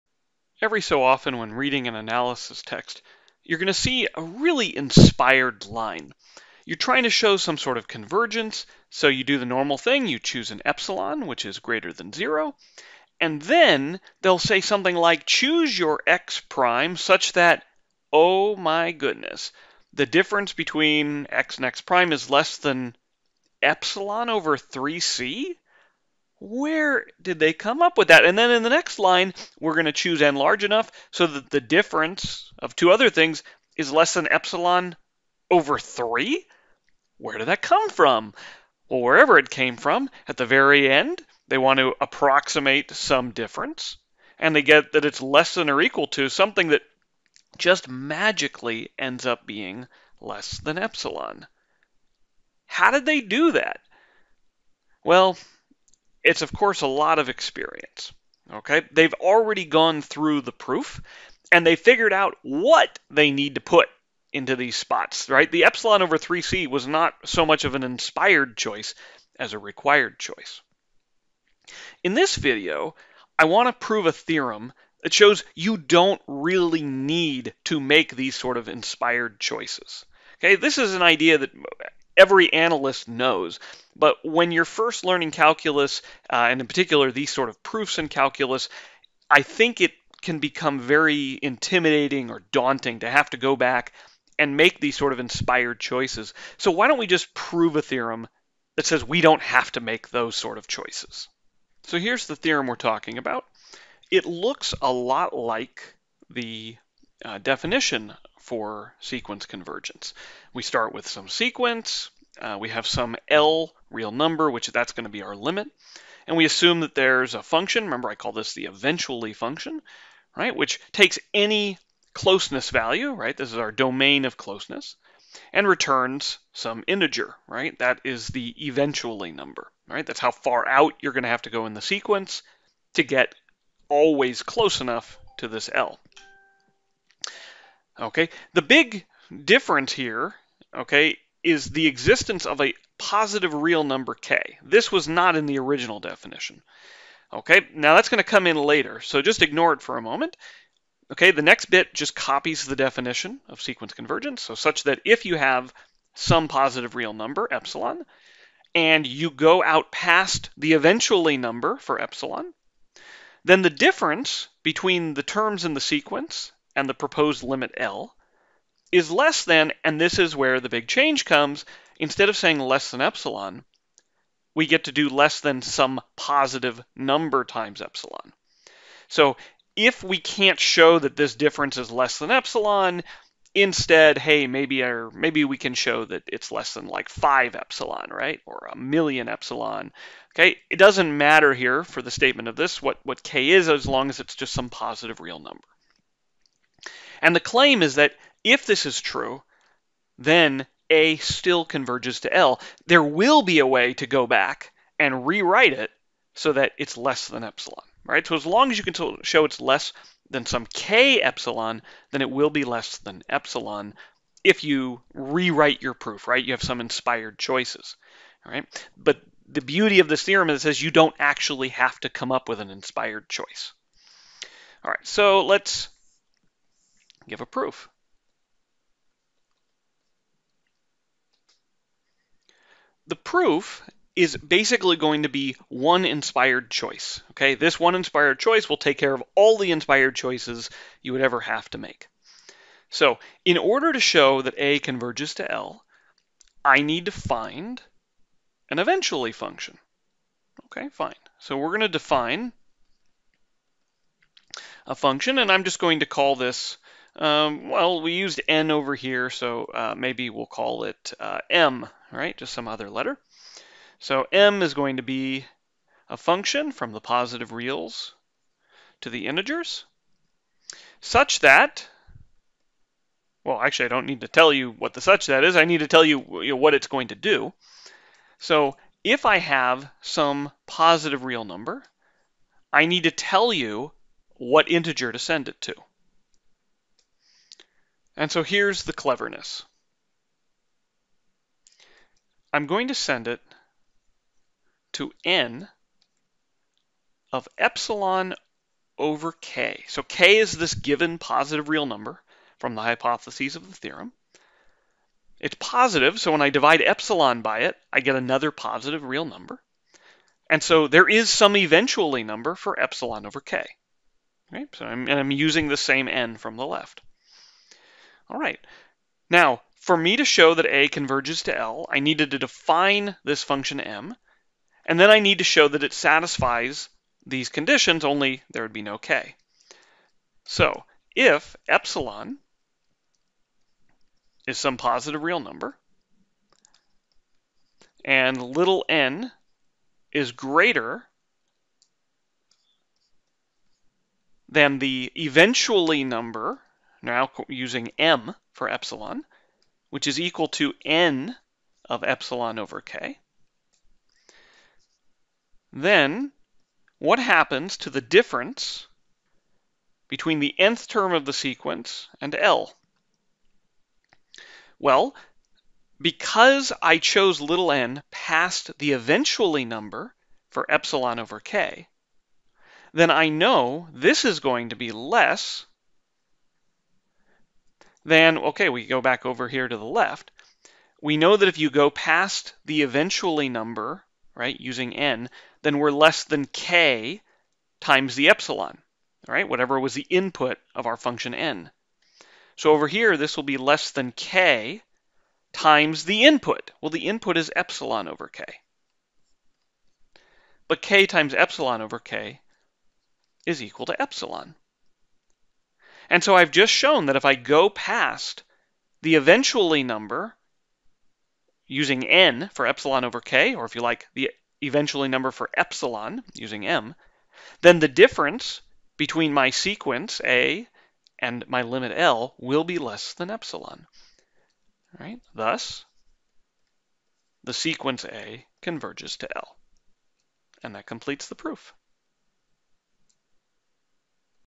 Proof of the Inspired Choice Theorem
In this video, we prove a profound, yet trivial result, one which is known intuitively by experts, but rarely written down. Simply, we show that when proving a sequence converges, it is enough to show that the terms in the sequence get within a positive multiple of close, rather than needing to be close. The title is somewhat sarcastic as the point of the result is to obviate the need for making as many inspired choices in convergence proofs.

The opening image is Proposition 5.17 from Gerald B. Folland's Real Analysis: Modern Techniques and Their Applications (Second Edition), the textbook I used for my analysis classes during my first year of graduate school.